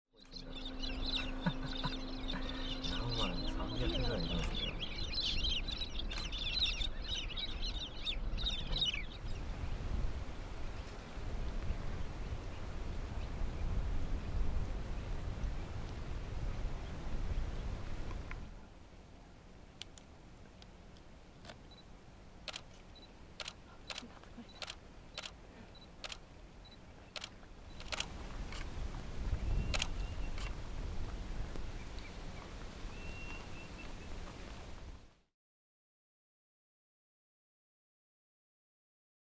野生のミドリマキエインコ【AAK Nature Watch】
マキエゴシキインコには多くの亜種があり、クィーンズランド州北西部の個体群は独特の淡い色合いをしており鳥の分類によっては独立種Cloncury Parrot（ミドリマキエインコ）として扱われる。